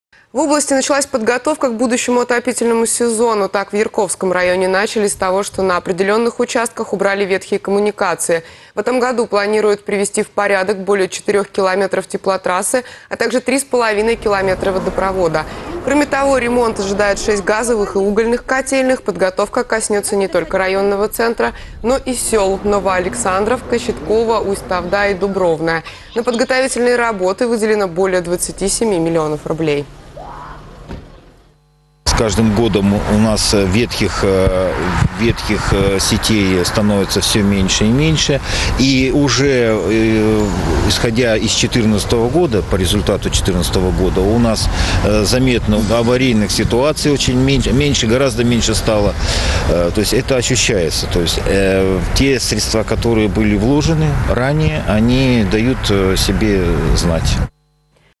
В Ярковском районе приведут в порядок более 4 км теплотрассы и 3,5 км водопровода
В  Тюменской области стартовала подготовка у будущему отопительному сезону. Так, в Ярковском районе начали с того, что на определенных участках убрали ветхие коммуникации. В этом году планируют привести в порядок более четырех километров теплотрассы, а также 3,5 км водопровода. Кроме того, ремонт ожидает шесть газовых и угольных котельных. Подготовка коснётся не только районного центра, но и сёл Ново-Александровка, Щетково, Усть-Тавда и Дубровное. На подготовительные работы выделено более 27 млн рублей.